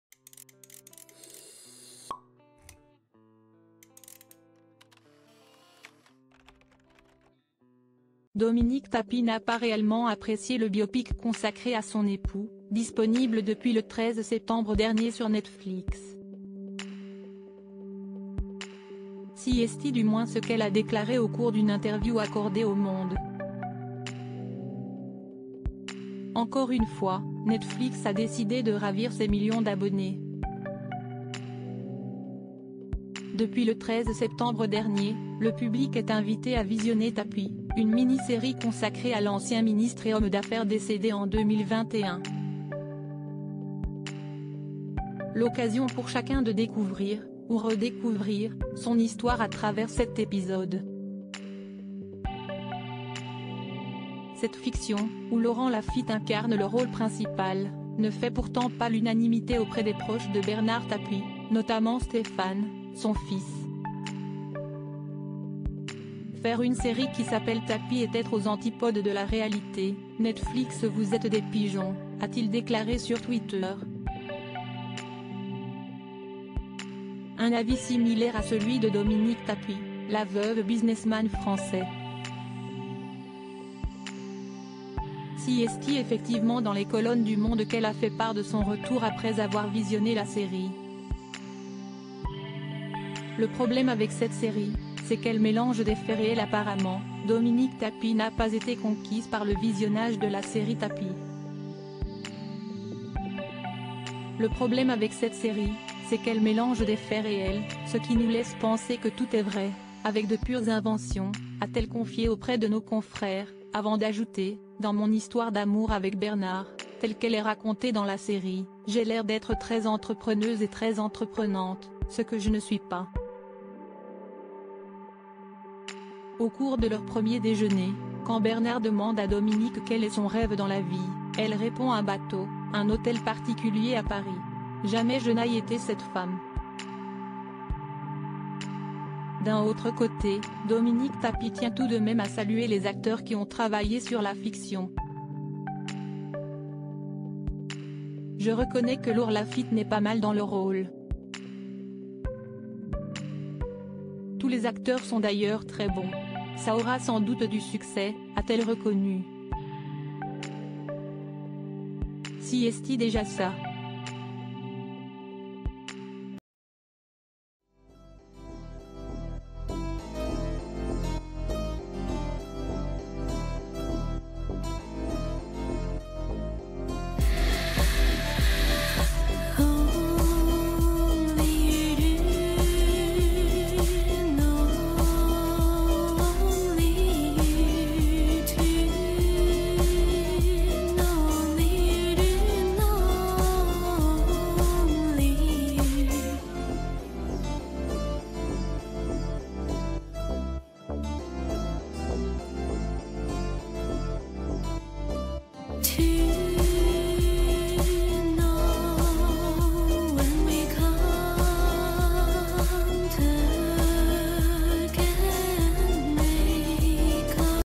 « Jamais je n'ai été cette femme ! » l'avis tranché de Dominique Tapie sur le biopic de Bernard Ta
« Jamais je n'ai été cette femme ! » : l'avis tranché de Dominique Tapie sur le biopic de Bernard Tapie